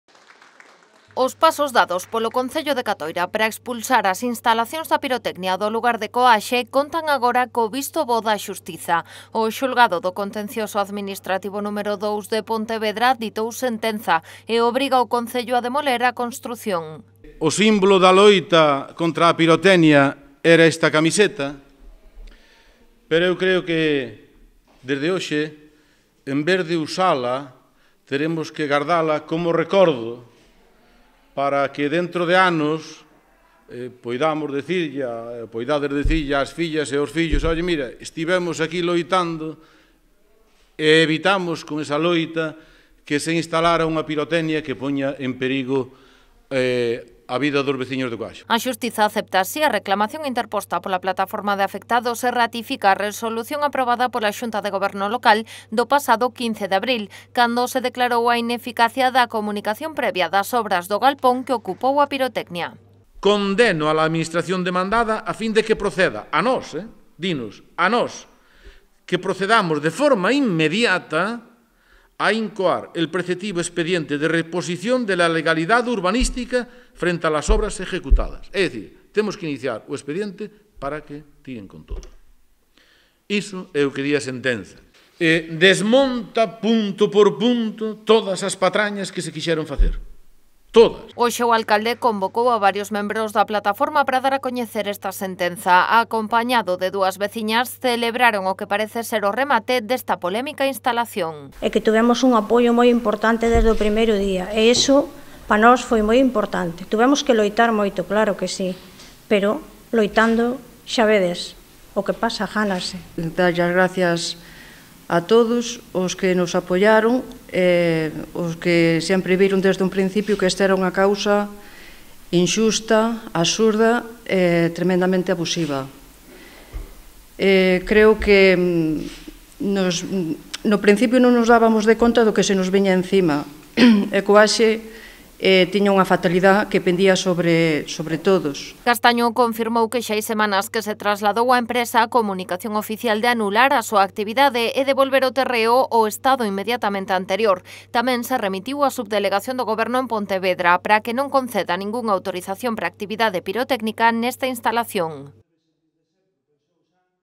A xustiza avala os pasos dados por Catoira contra a pirotecnia de Coaxe e obriga á súa demolición
As instalacións da pirotecnia de Coaxe, no concello de Catoira, terán que desaparecer. O xulgado do Contencioso Administrativo número 2 de Pontevedra dá a razón á plataforma de persoas afectadas e avala os pasos dados polo goberno catoirense. A sentenza declara a ineficacia da comunicación previa das obras e obriga á demolición da construción de maneira inmediata.